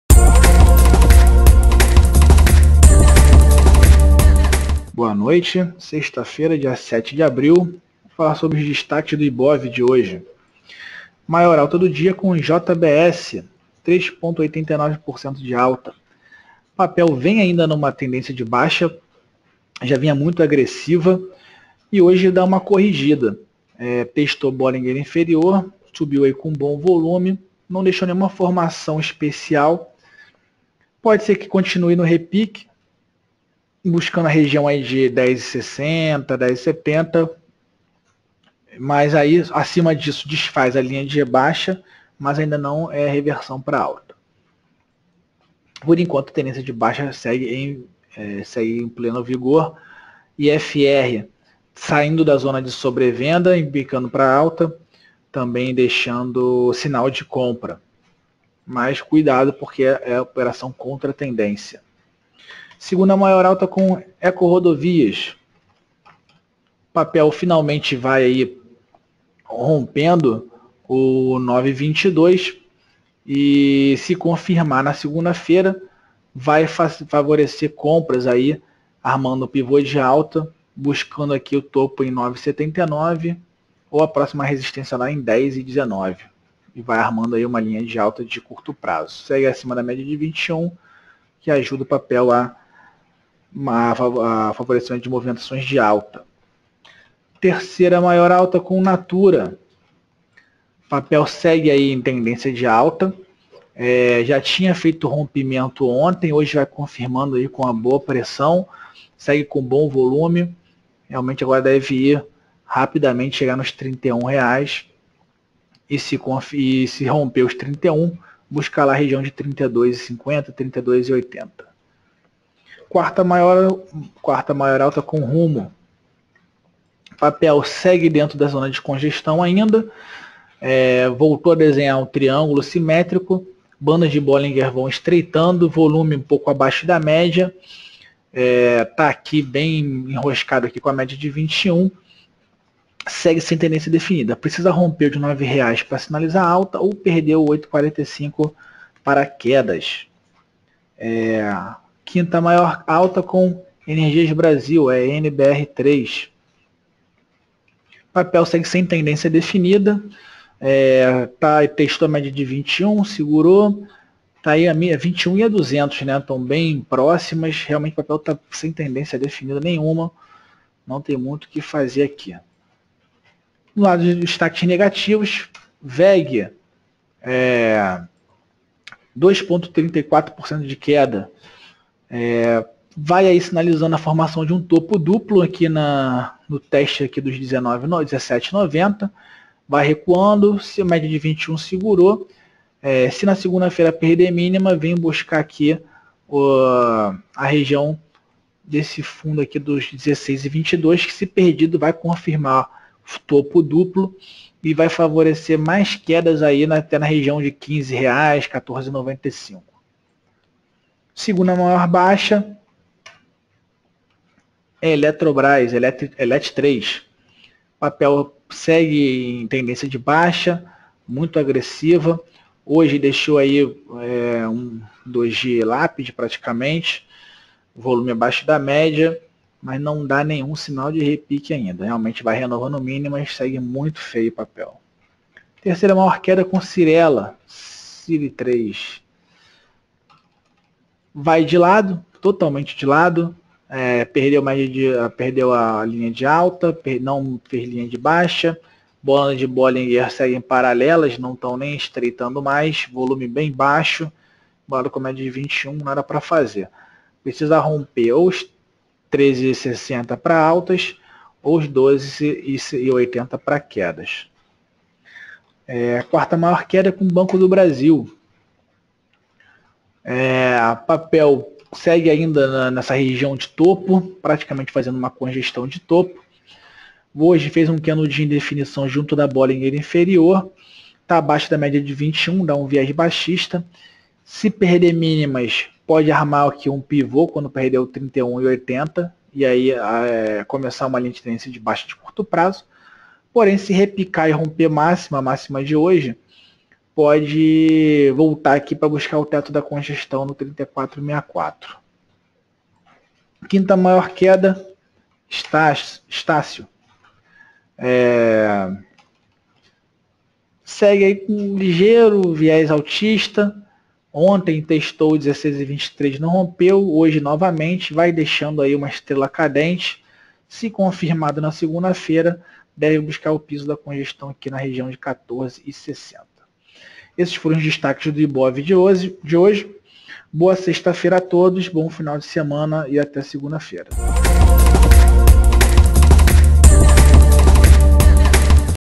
Resumo do Dia MyCAP 07/04/2017
O Ibovespa brasileiro subiu 0,58% para 64,593 pontos, mas deu alguns dos ganhos anteriores depois que o governo anunciou um corte para a meta fiscal de 2018, de US $ 79 bilhões para US $ 129 bilhões. O volume total hoje foi de R $ 6,6 bilhões.

Os melhores resultados foram: JBSS3 (+4,21%), ENBR3 (+ 2,51%), USIM5 (+ 2,03%), CSAN3 (+ 1,92%) e ECOR3 (+ 1,89%).

Os maiores declinadores do índice foram: WEGE3 (-2,63%), TIMP3 (-1,47%), ELET3 (-1,31%), ESTC3 (-1,23%) e SUZB5 (-1,18%).